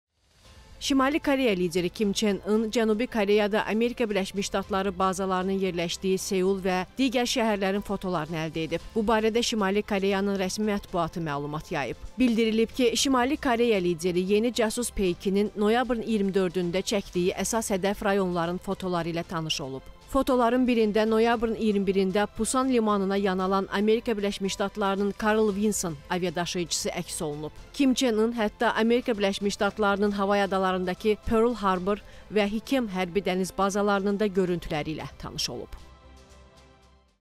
Kim Çenin ABŞ-də əsas hədəfi: gizli görüntüləri ələ keçirdi
Şimali Koreya lideri Kim Çen İn Cənubi Koreyada ABŞ bazalarının yerləşdiyi Seul və digər şəhərlərin fotolarını əldə edib.
Bu barədə Şimali Koreyanın rəsmi mətbuatı məlumat yayıb.
Bildirilib ki, Şimali Koreya lideri yeni casus peykinin noyabrın 24-də çəkdiyi əsas hədəf rayonların fotoları ilə tanış olub. Fotoların birində noyabrın 21-də Pusan limanına yan alan ABŞ-nin “Karl Vinson” aviadaşıyıcısı əks olunub.
Kim Çen İn hətta ABŞ-nin Havay adalarındakı Pyorl-Harbor və Hikem hərbi-dəniz bazalarının da görüntüləri ilə tanış olub.